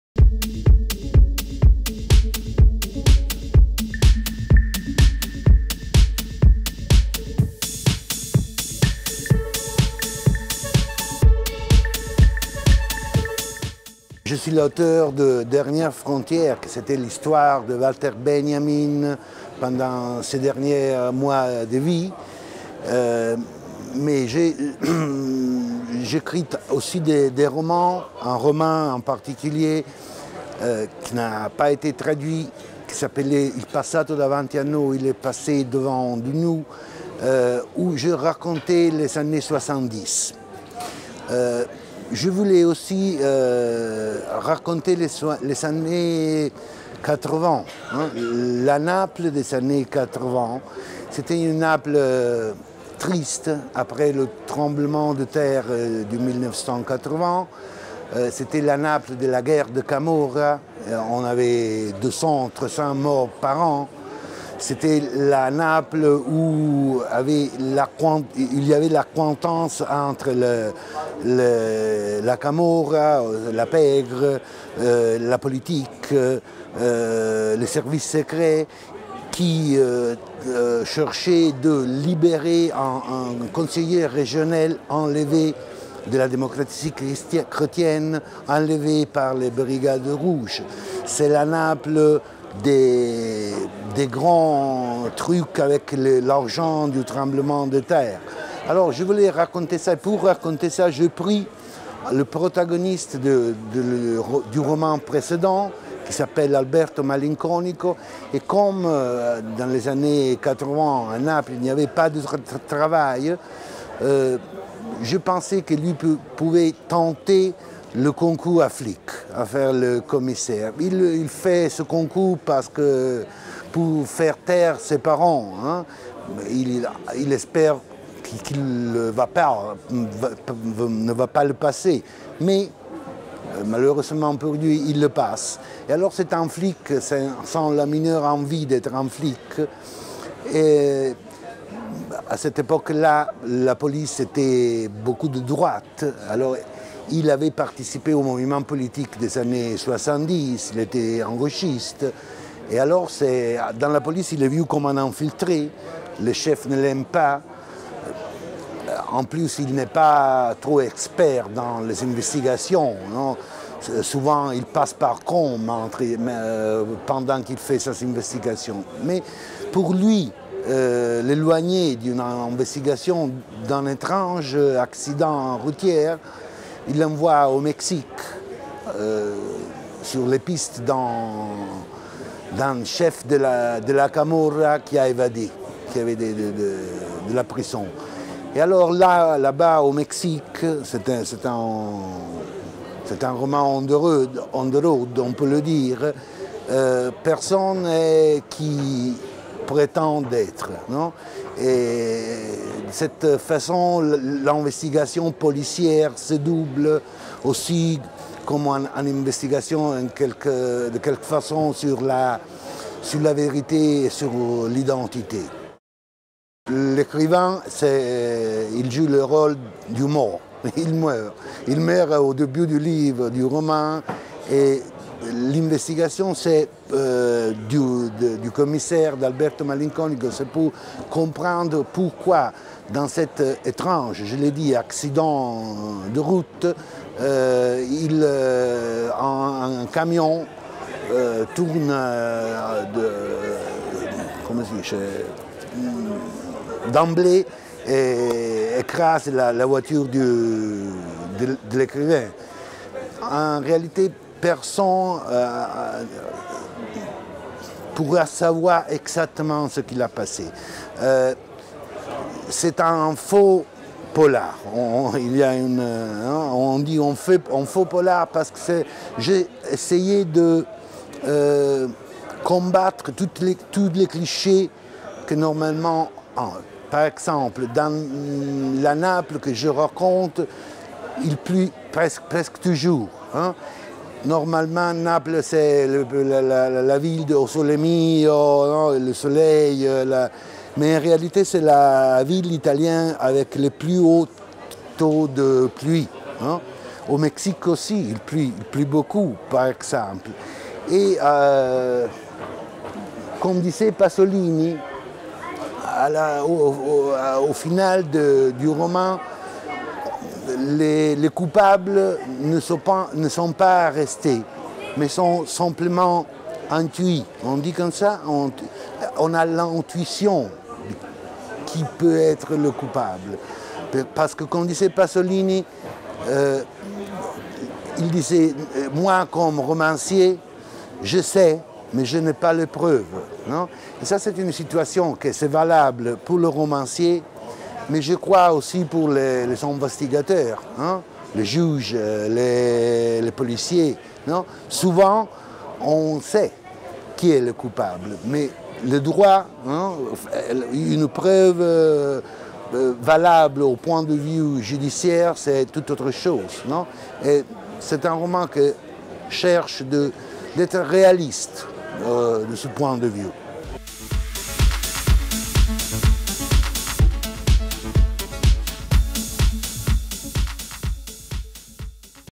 Bruno Arpaia - Avant la bataille
A l'occasion de l'Escale du Livre 2015 à Bordeaux, Bruno Arpaia vous présente son ouvrage "Avant la bataille" aux éditions Liana Levi. Traduit de l'italien par Fanchita Gonzalez-Batlle.